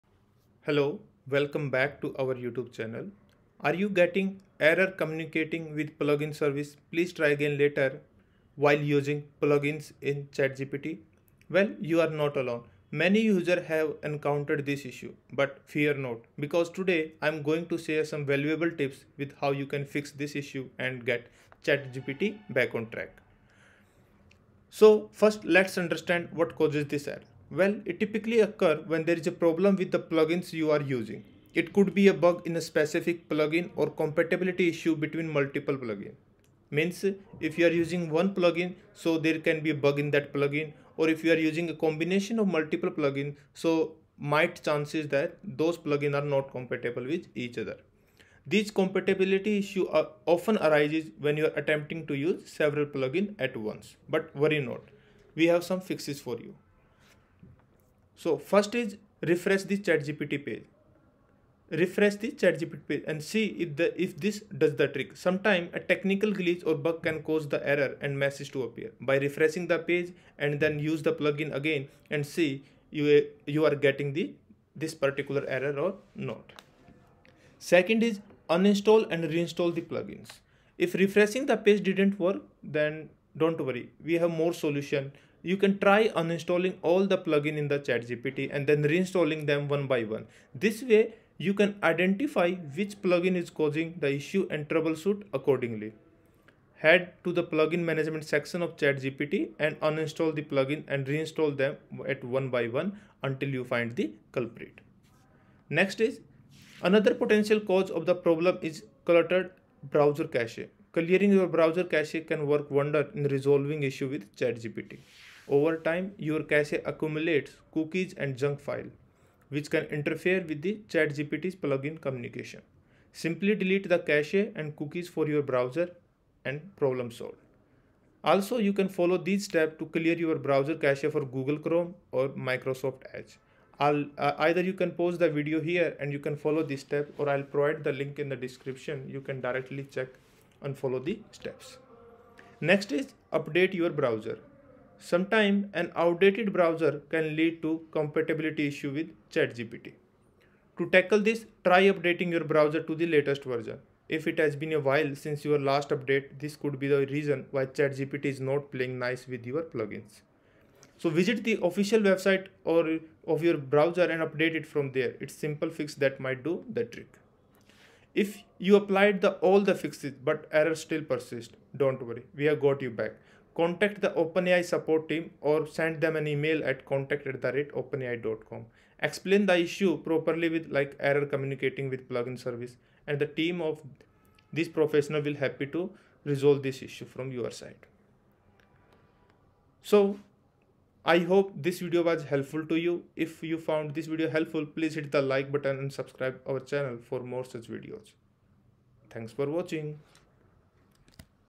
How to Fix "Error Communicating With Plugin Service Please Try Again Later" on ChatGPT
Are you frustrated with the "Error communicating with plugin service, please try again later" message while using plugins in ChatGPT? Well, you're not alone! Many users have encountered this issue. But fear not, because today, I'm going to share some valuable tips with you on how to fix this problem and get your ChatGPT back on track.

00:00 Intro
00:27 What Causes “Error Communicating With Plugin Service, Please Try Again Later” Error On ChatGPT?
01:00 Solution 1: Refresh The ChatGPT Page
01:18 Solution 2: Uninstall & Reinstall The Plugins
01:46 Solution 3: Clear Your Browser Cache
02:23 Solution 4: Update Your Browser
02:48 Solution 5: Contact The OpenAI Support Team
03:11 Conclusion